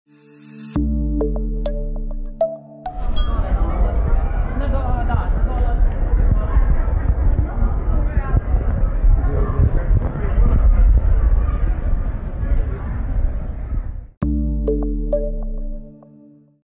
Peđa Medenica PRVI PUT U JAVNOSTI SA PORODICOM nakon skandala sa rijaliti učesnicom!
Pevača Peđu Medenicu paparaco "Blica" danas je prvi put uhvatio sa suprugom i decom, nakon skandala sa rijaliti učesnicom Jovanom Cvijanović.